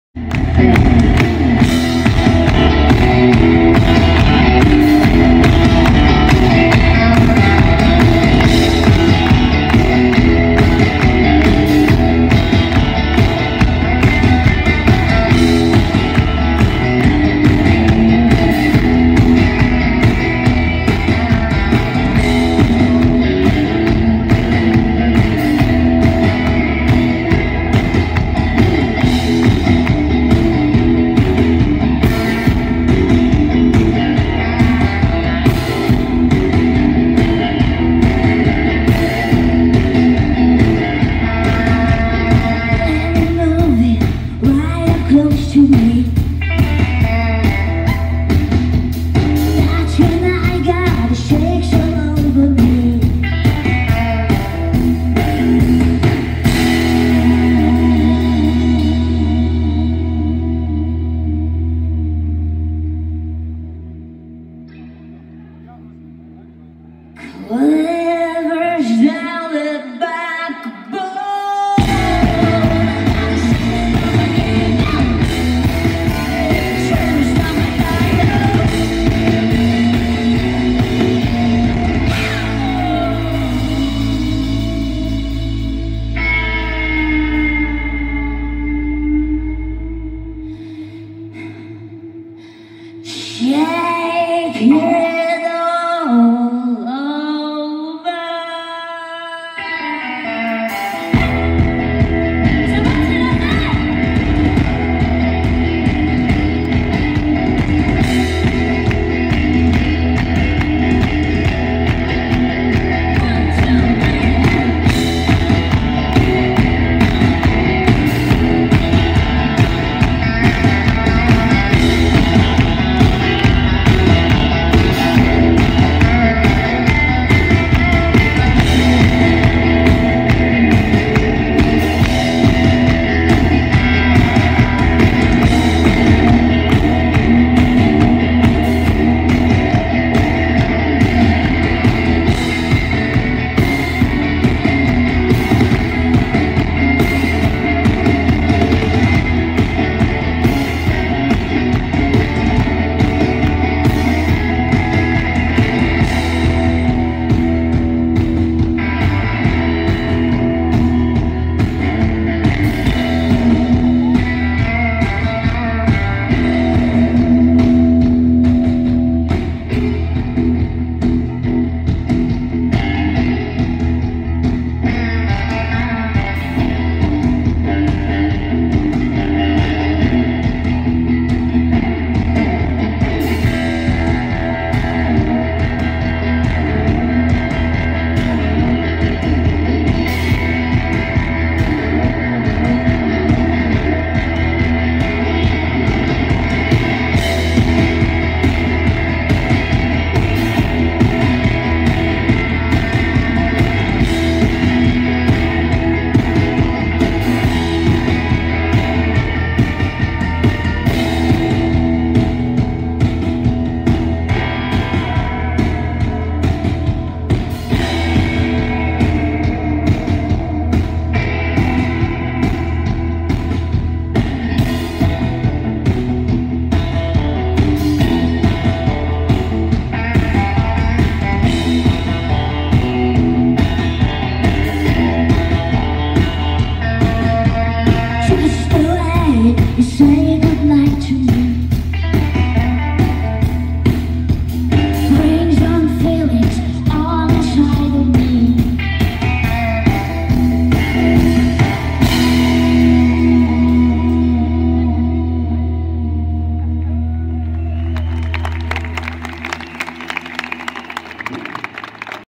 The Blind Suns - Trélazé (Live)
This video of The Blind Suns performing live on stage at the Festival de Trélazé, close to Angers, this July 2019.

The Blind Suns are a Polish-French group comprising of Romain Lejeune on Guitar, Dorota Kuszewska singing and Josselin Matignon on Drums.  Singing in English, they have found international success performing in the USA, Poland, Asia, Russia, Italy, Germany, Switzerland as well as France.

This is just one of the songs that the Blind Suns performed on stage at the  parc du Vissoir in Trélazé, close to Angers in the Maine-et-Loire department of the Pays de la Loire in France.

Support us?